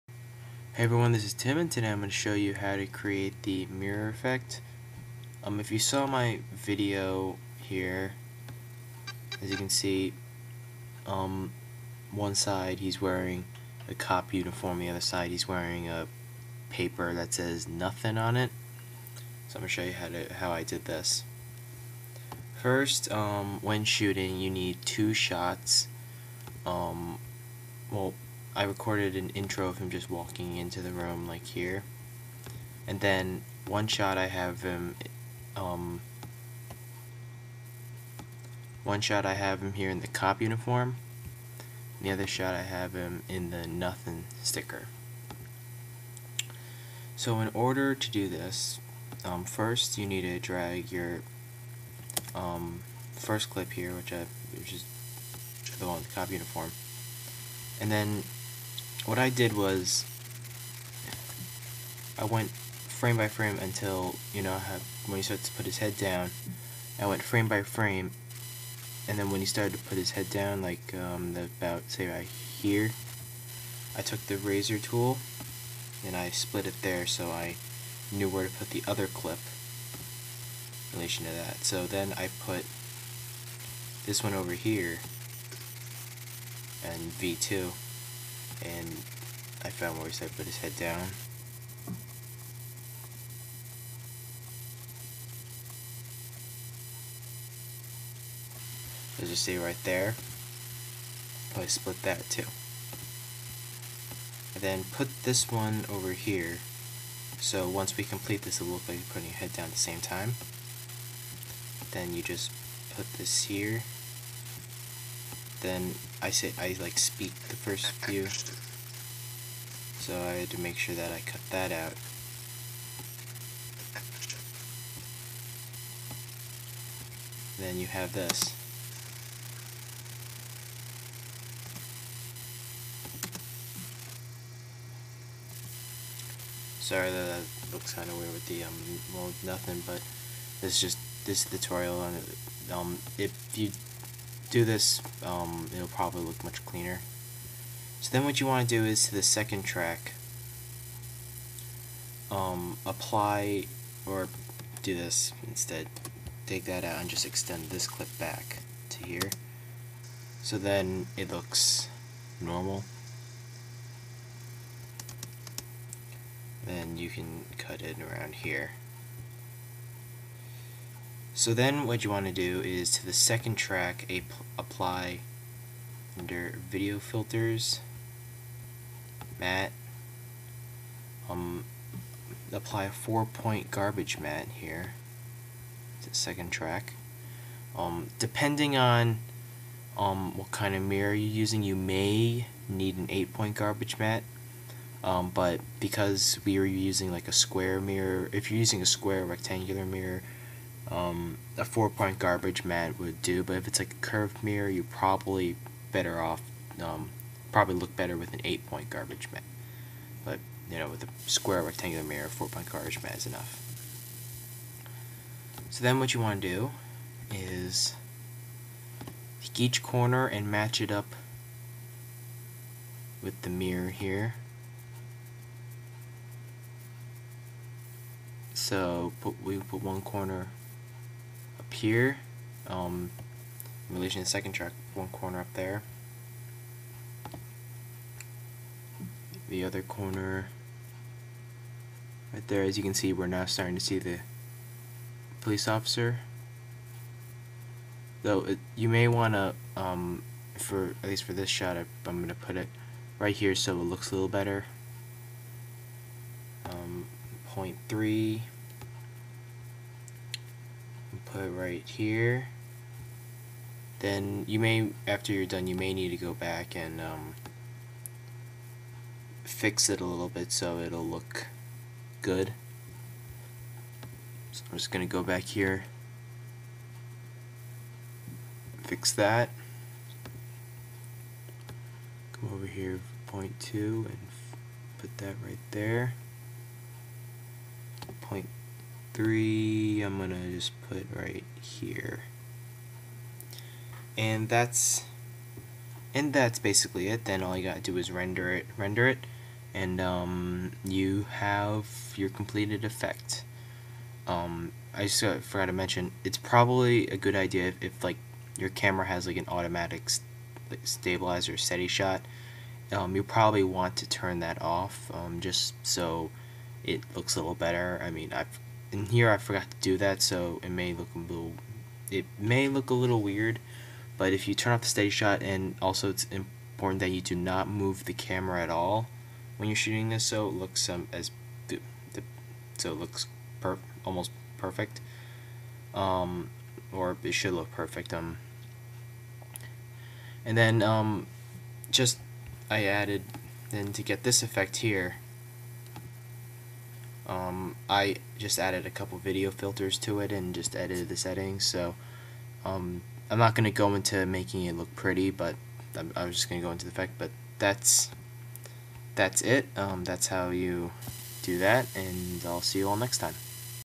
Mirror Tutorial
This is my tutorial for the dual reflection effect.

(This tutorial is meant for Final Cut Express/Pro users)

Tags: calc productions mirror reflection tutorial livetype final cut express 4 final cut pro.